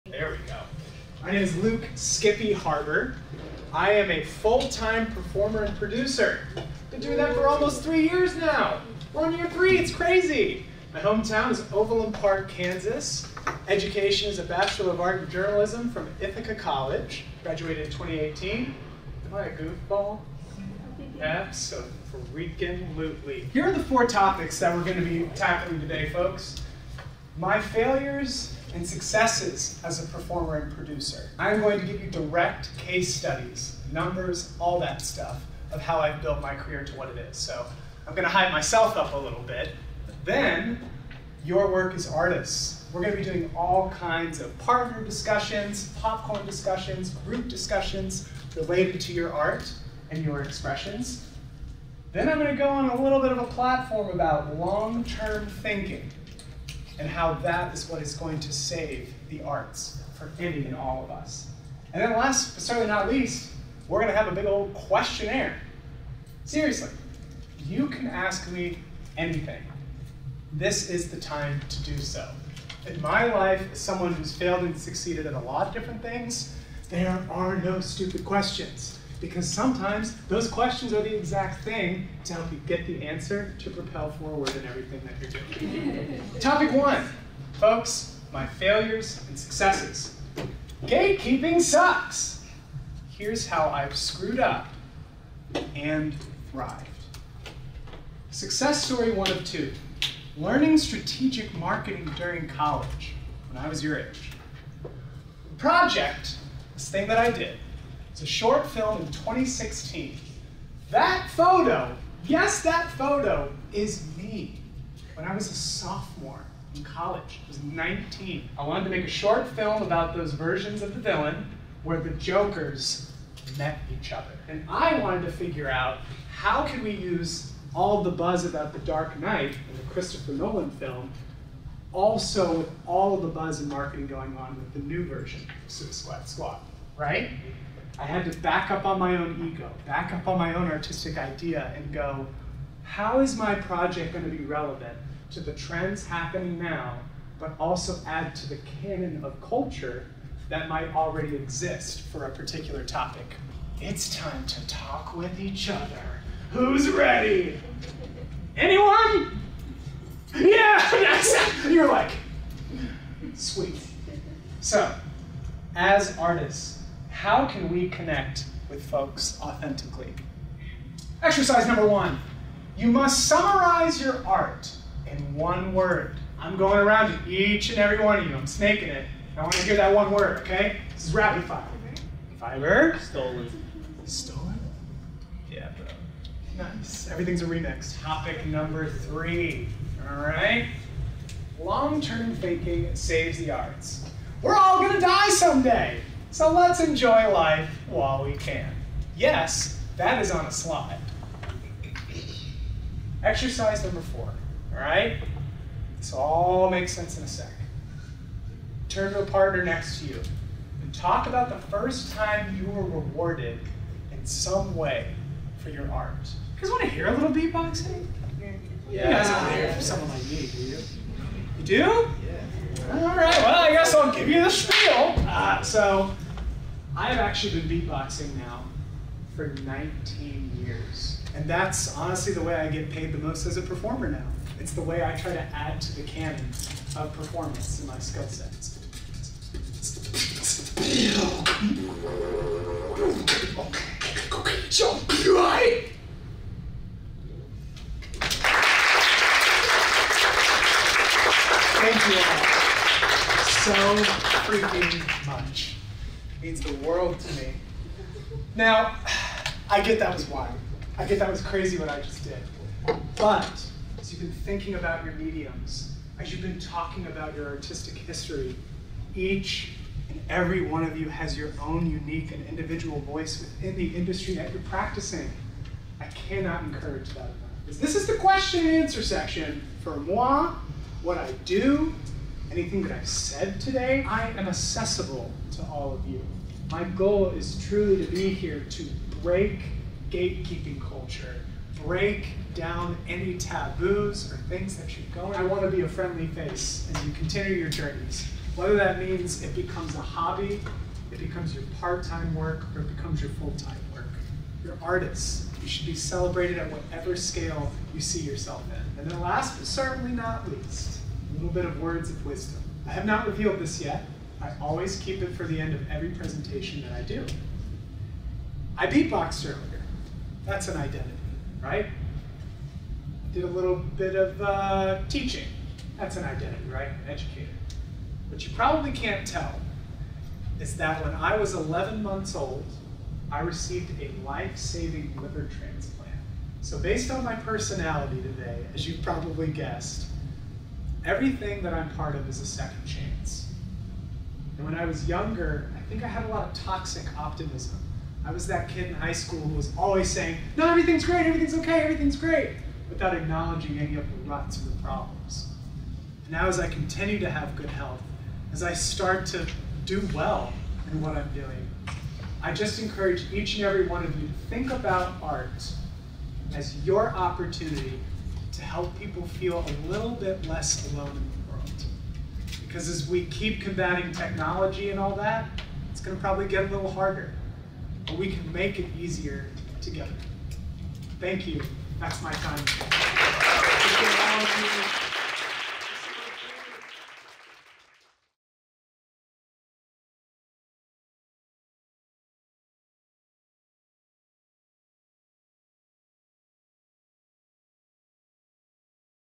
Luke “Skippy” Harbur @ Kansas City Art Institute
On March 21, 2024 Luke “Skippy” Harbur hosted a 75-minute guest lecture on arts entrepreneurship at the Kansas City Art Institute, focusing on topics of his own arts business failures and successes, community building, and identity.

The full lecture includes multiple case studies on arts business failures and successes, multiple opportunities for one-on-one and room-wide discussions, and an unfiltered opportunity for questions and answers about Luke "Skippy" Harbur's artistic journey.

To get in touch with Harbur to speak at your institution/organization, head